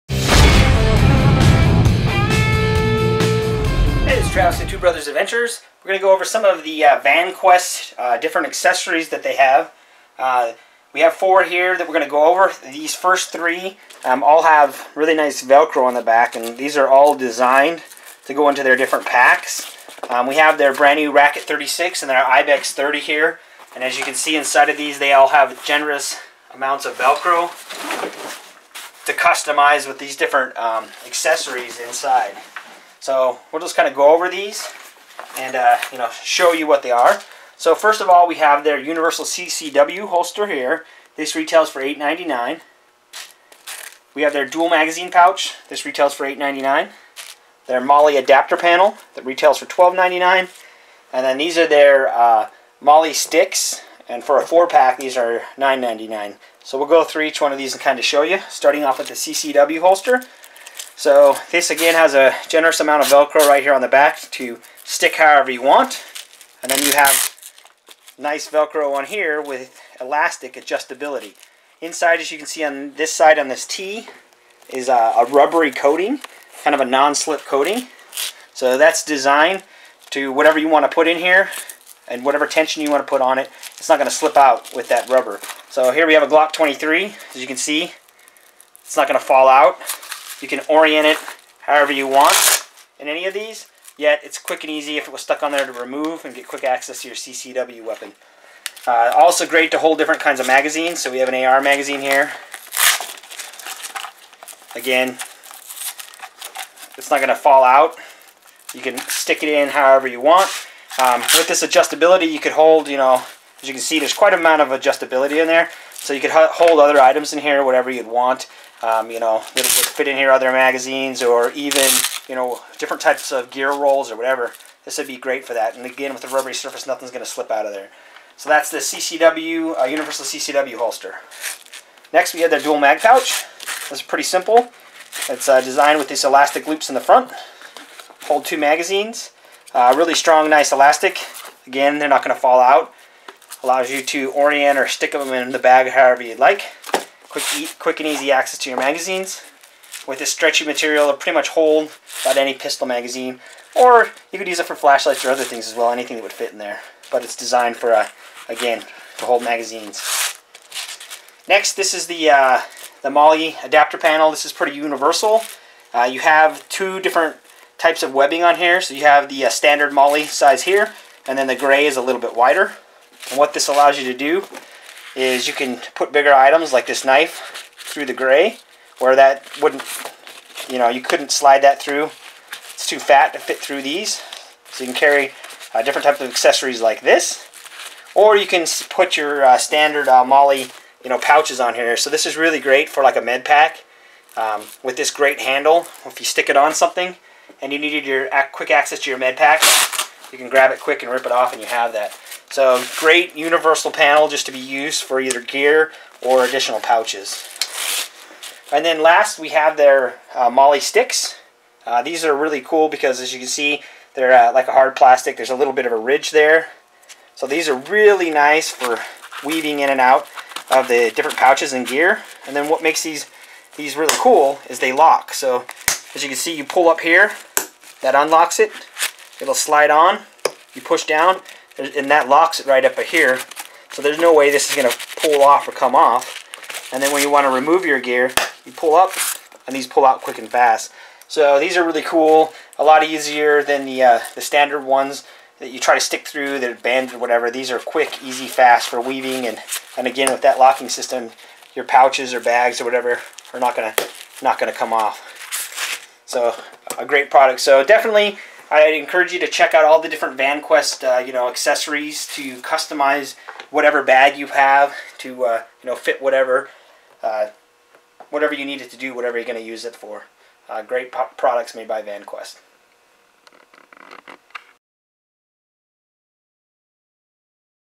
VANQUEST Accessories
We give you a look at some of the available VANQUEST accessories.  We give you a look at the Universal CCW Holster, Mohl Adapter Panel, Double Mag Holder and Molle Sticks.  These quality accessories let you customize your VANQUEST gear bags (or any brand bag with hook and loop patches) to fit your needs and mission specifications.